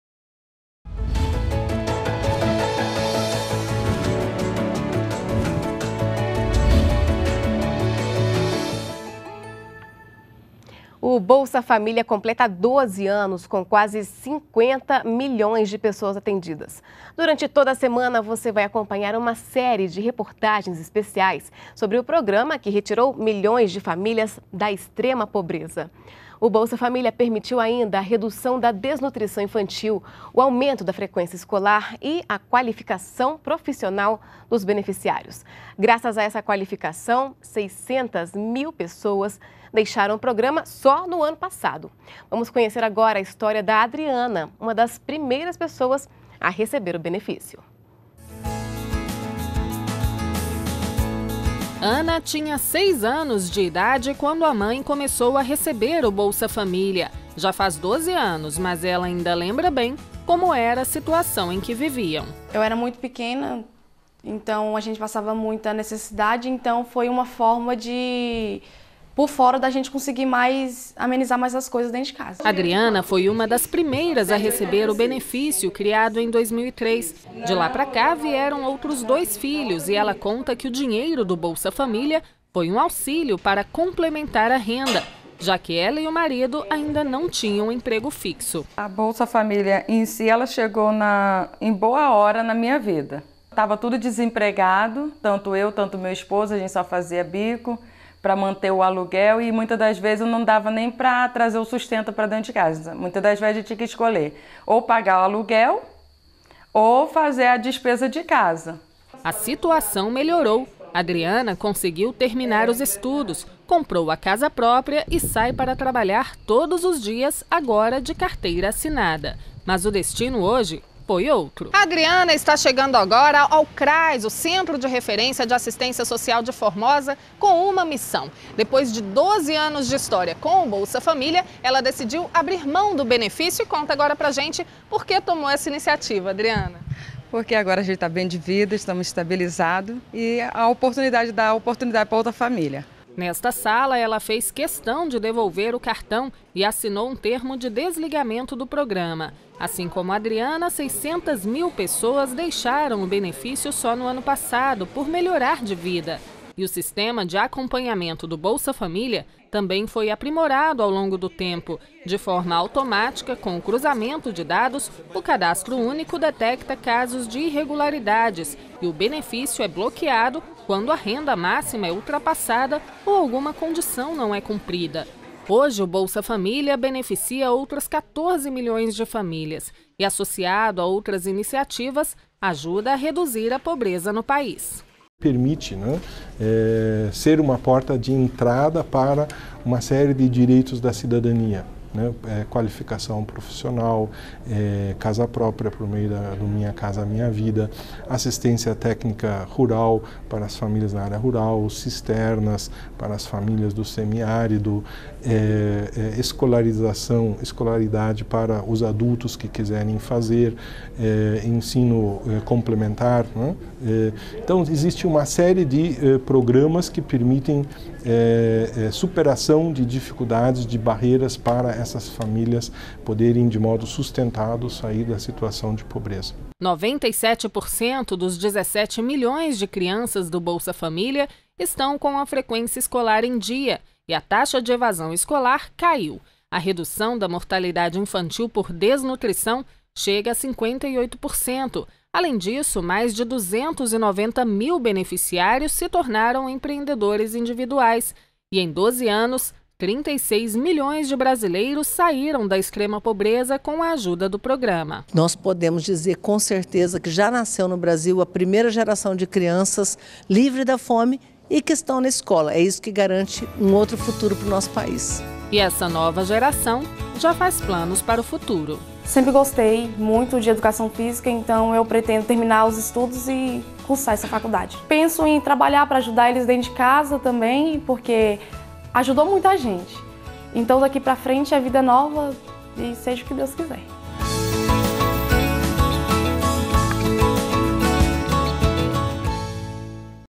Bolsa Família é o maior programa de transferência de renda do mundo
NBR NOTÍCIAS - 19.10.15: O programa Bolsa Família foi lançado em 2003 e atende famílias que vivem em situação de pobreza e extrema pobreza, com renda mensal de até R$ 77 por pessoa. O programa brasileiro completa 12 anos e é considerado o maior programa de transferência de renda do mundo, além de ter ajudado o Brasil a sair do Mapa Mundial da Fome. São atendidas atualmente mais de 13,9 milhões de famílias em todo o país.